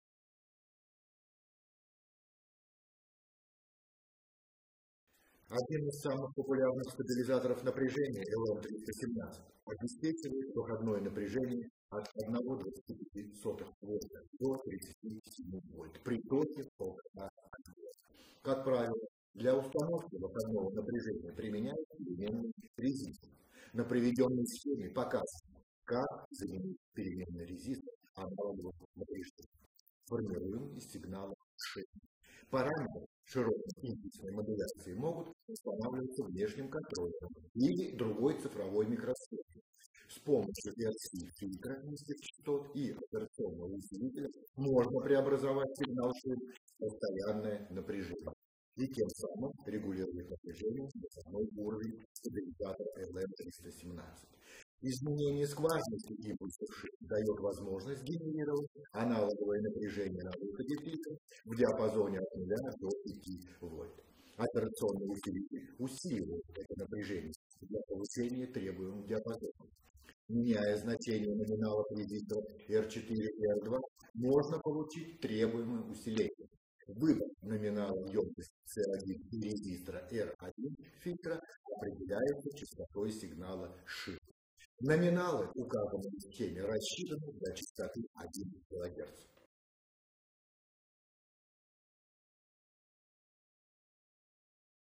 Control of LM317T PDM Stabilizer
Control of LM317T PDM Stabilizer       LM317 is one of the most widely used voltage stabilizers. It provides 1.25V - 37V output voltage фтв 1.5А current. Generally, a potentiometer is used to adjust the output voltage.       The diagram demonstrates use of the analog voltage as a potentiometer generating PDM signal. PDM parameters may be set by an external controller or any other digital integrated circuit.       PDM signal may be converted into DC voltage by RC low-frequency filter and an operating amplifier. It provides voltage adjustment of LM317 stabilizer output level.       Alteration in relative PDM pulse duration provides generation of 0V -- 5V analog voltage at the filter output.       Operating amplifier increases the voltage to a required rate. Alteration in R4 and R2 resistor ratings provides required amplification. Capacity of С1 and R1 filter resistor depends on PDM signal frequency. The diagram includes rates for a frequency of 1kHz.